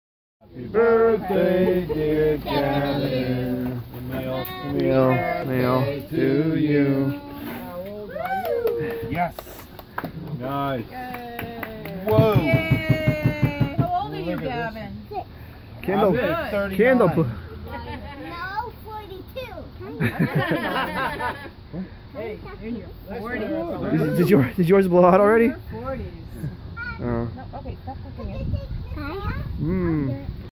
874 - Happy 6th Birthday Gavin
It was Father's Day, which happened to be the same day as Gavin's birthday. (Also, my brother Neil had his b-day a couple days later.) ...